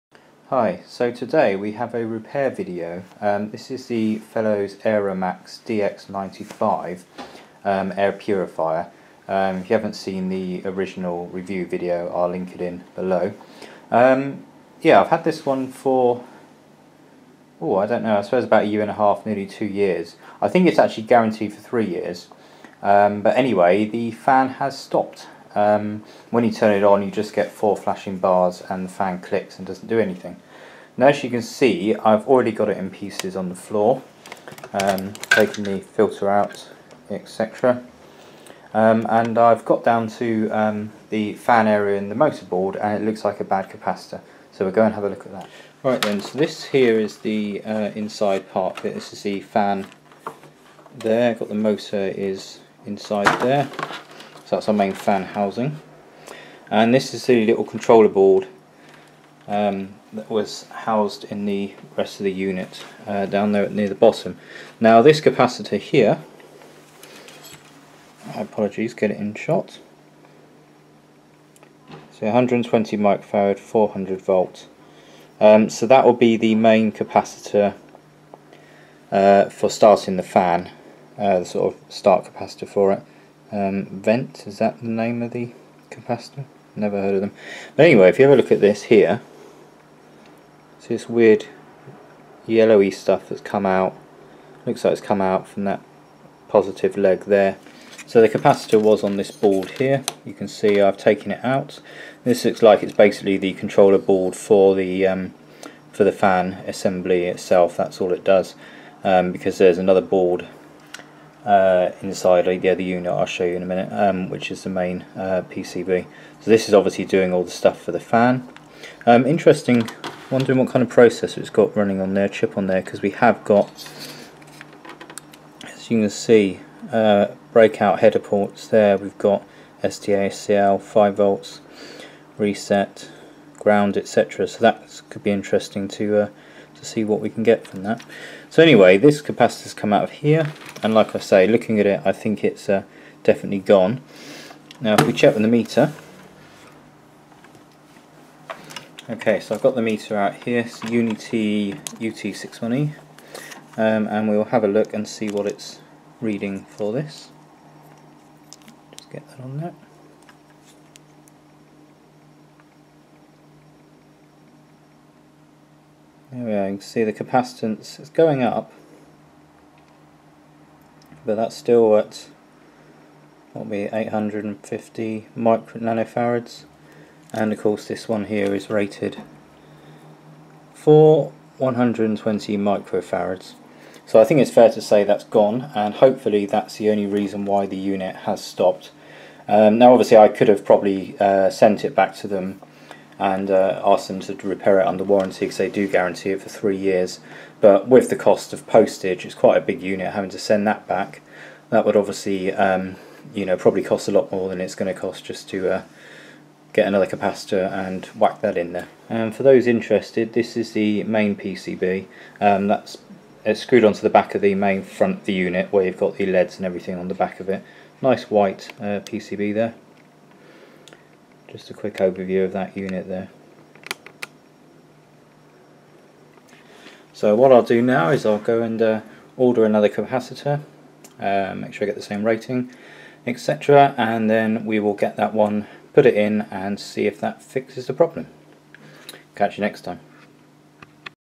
Fellowes AeraMax DX95 Repair - Part 1
In Part 1 - I suspect i've found the cause of the fan not working on this air purifier, Could it be a bad cap?